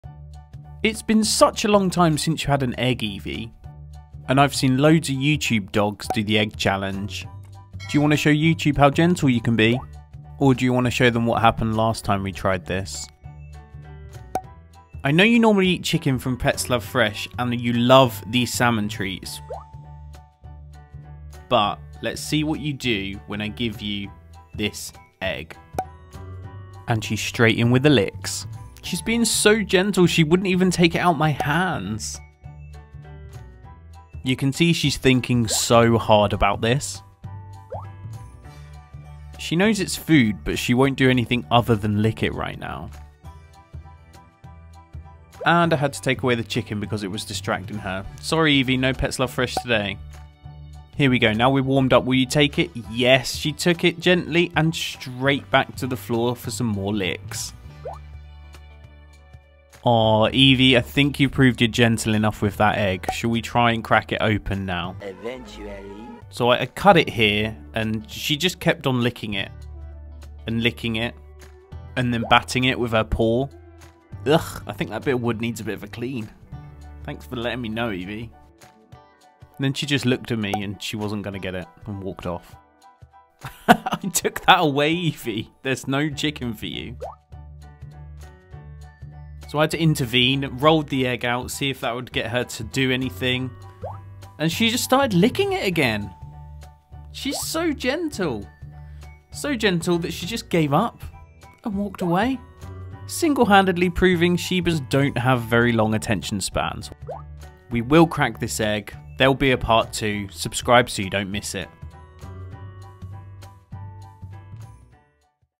Testing my Dog: Gentle or Crazy?
SHIBA does the EGG CHALLENGE! My Shiba Inu dog Eevee Bork takes on THE EGG CHALLENGE! My shiba is a very gentle princess and licks the egg in this challenge so much it frustrates her and she acts stropy and walks off. Eevee is a calm Shiba Inu with plenty of sass in her personality so watching how she licks and handles the egg makes me FEEL her tension rise xD

In this video you'll see some Pets Love Fresh cooked chicken that Eevee enjoys for dinner and training.

Facts about Eevee the Shiba Inu (eevee bork):
Name: Eevee
Age: 7
Gender: ♀
Fur color: Red
Birthday: Feb 12th

00:00 Intro
00:24 Egg Delivery 1
00:51 Egg Delivery 2
01:28 Egg delivery 3
01:42 Quitting or??